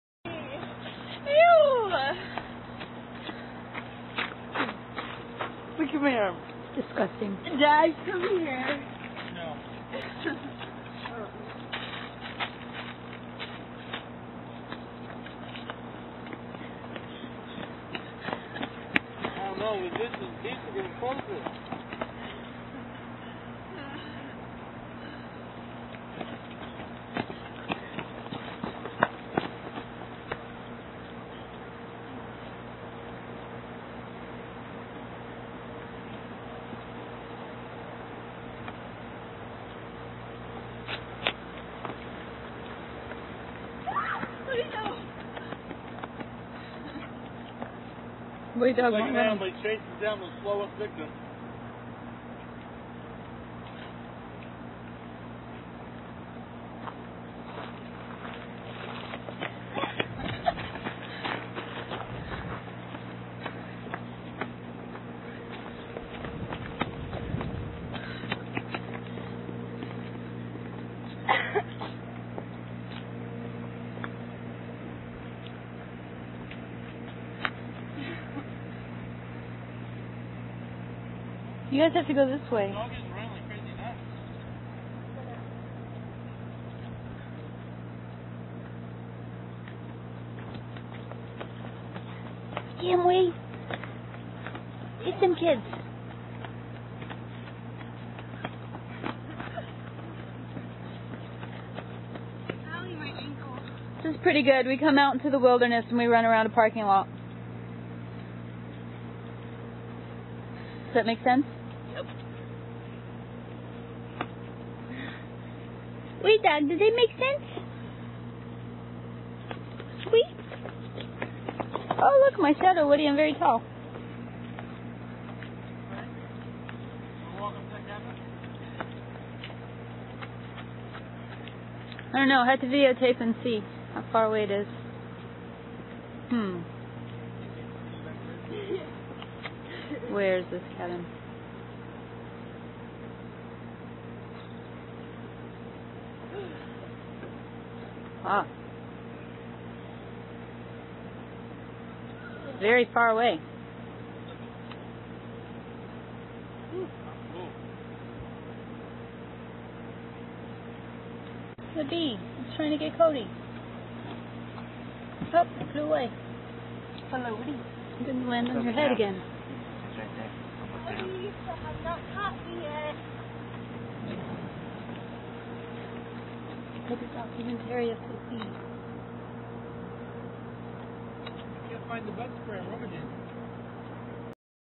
Cody and Cheyenne chasing each other at Independence Mine
Hatcher Pass and Independence Mine.